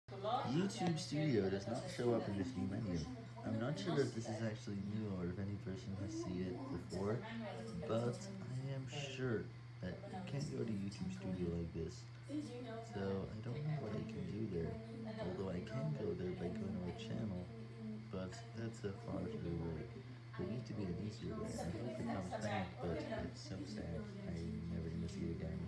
YouTube Studio is not in the new menu.
I noticed it, but is it new.